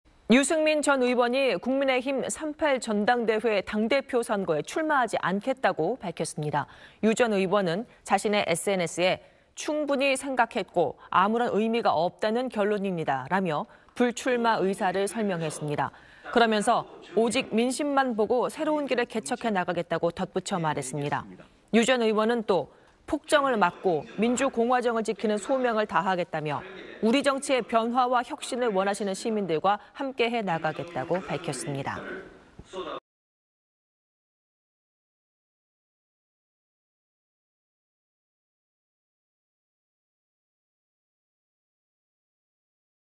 유승민, 당 대표 불출마…"의미 없다는 결론…새 길 개척하겠다" / SBS
유승민 전 의원이 국민의힘 3·8 전당대회 당 대표 선거에 출마하지 않겠다고 밝혔습니다.

유 전 의원은 자신의 SNS에 "충분히 생각했고, 아무런 의미가 없다는 결론입니다"라며 불출마 의사를 설명했습니다.

그러면서 "오직 민심만 보고 새로운 길을 개척해 나가겠다"고 덧붙여 말했습니다.

유 전 의원은 또 "폭정을 막고 민주공화정을 지키는 소명을 다하겠다"며 "우리 정치의 변화와 혁신을 원하시는 시민들과 함께 하겠다"고 밝혔습니다.